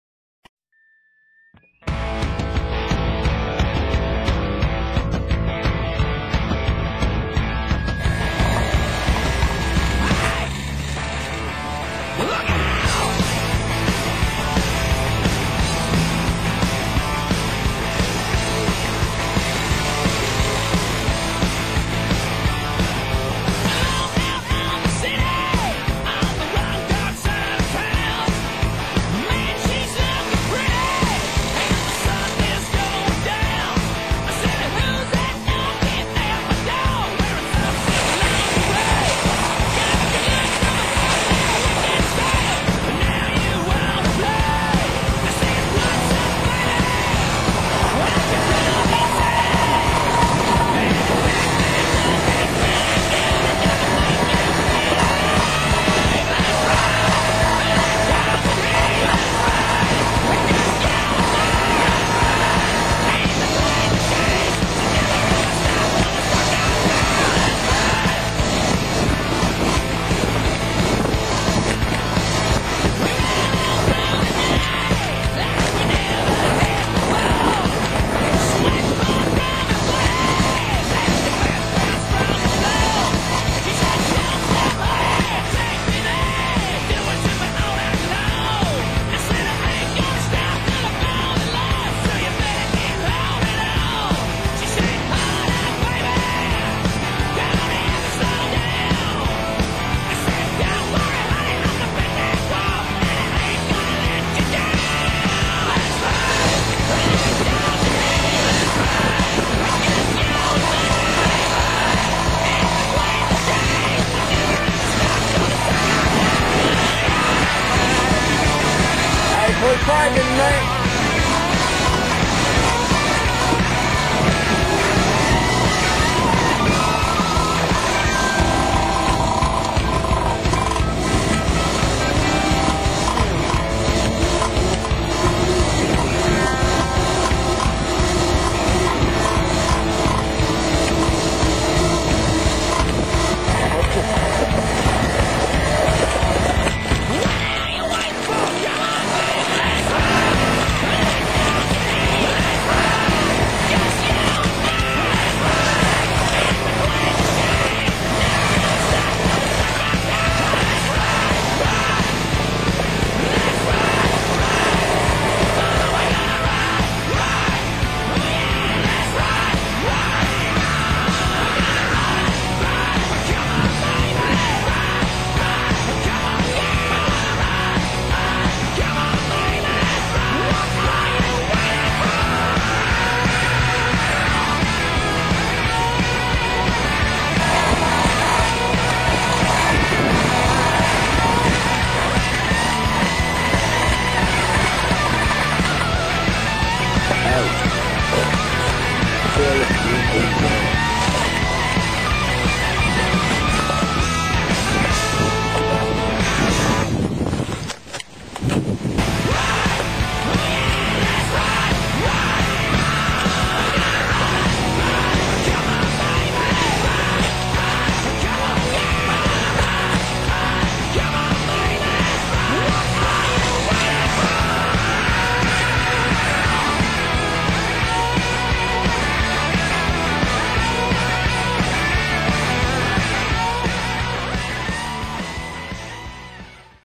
PRESENTATION EC500S LETS RIDE
Motocross Urbain

EC500

configurable pour la route avec mirroirs ,
 lumieres et pneu de route...
 ou original Hors route

SPÉCIFICATION:

Modeles : EC500s
Année : 2010
Voltage : 36 volt